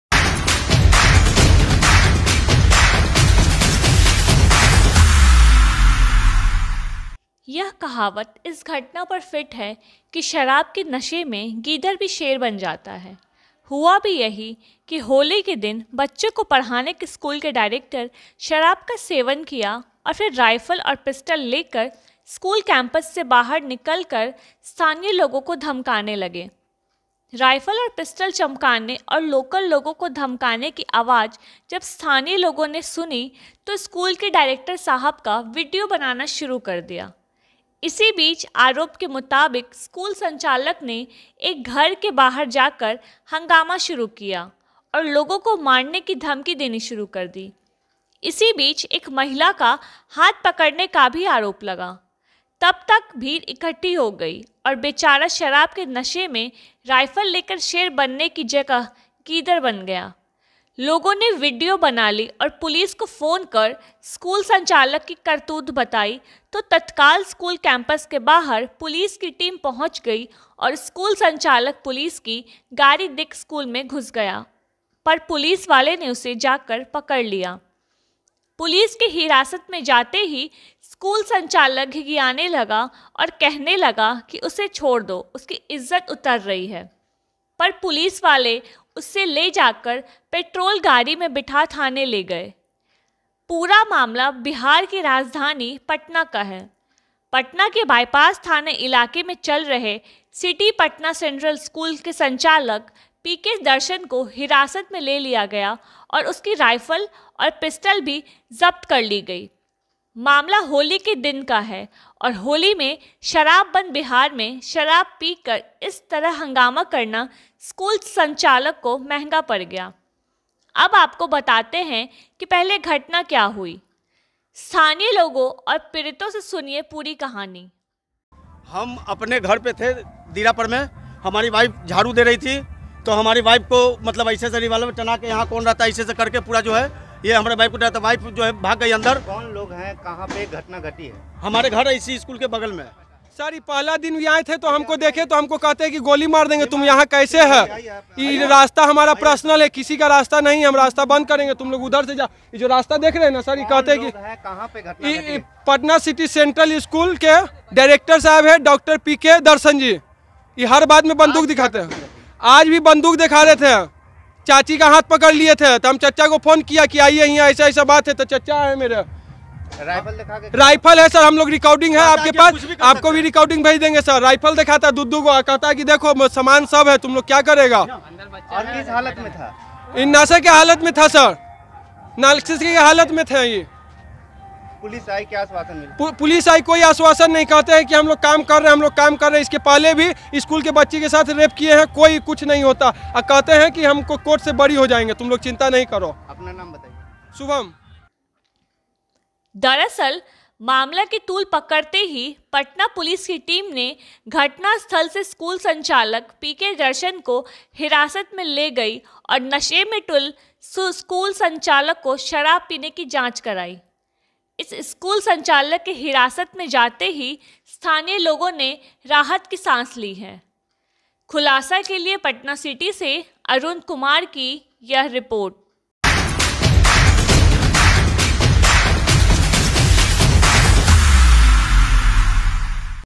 KhulasaMedia : school चलाने वाले को police ने Weapons के साथ किया गिरफ्तार
पटना सिटी से अरुण कुमार

यह कहावत इस घटना पर फिट है की शराब के नशे में गीदड़ भी शेर बन जाता है .

Here you will find Bihar News , Jhakhand News & Nepal News, National Politics, State Politics , Crime & Social issue